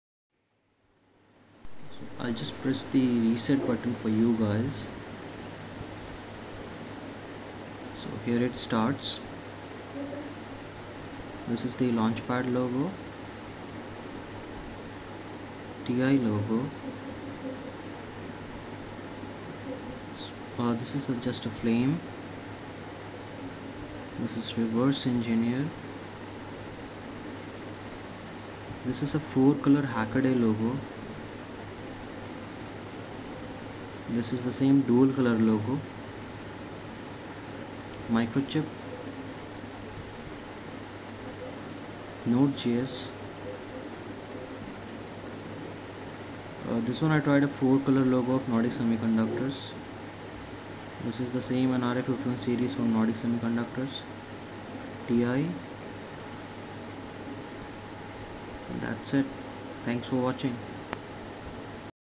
Displaying images on MSP430F5438
I have been trying to implement some games on the MSP430F5438 Experimenters board. My first stop was learning how to display bitmaps on the LCD screen. Here i am using the msp430 graphics library to display images.